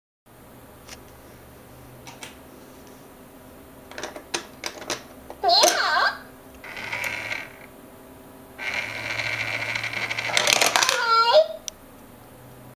Panda Saving Pot Coin Bank for Coin Collection
Main Features:
Creative battery operated panda coin bank.
This robotic panda will gladly steal out your spare change and save money for you.
Realistic movements and accepts virtually all coins.
A fun and safe way to save money.
Material: Non - toxic PVC.
Battery: 2 x AA battery (not included).